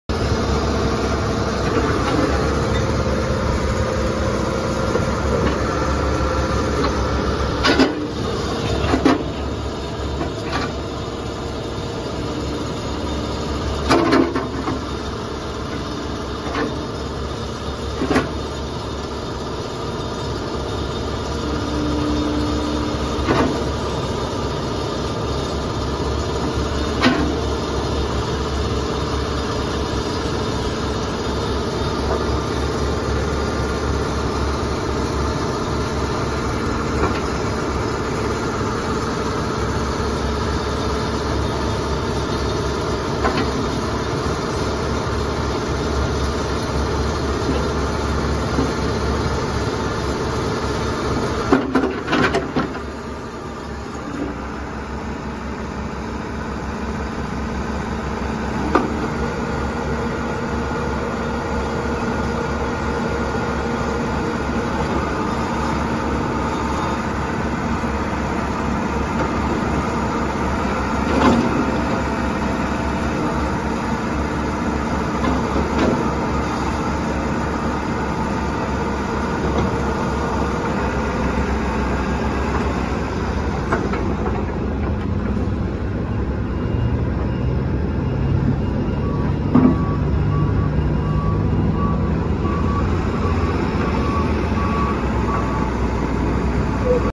CATERPILLAR 416D For Sale
4F/4R Power Shuttle Transmission, 75 HP Caterpillar Diesel, 14' Max Digging Depth, 8980 LBS Loader Breakout Force, Located at McGrew Equipment Company (Seven Valleys PA)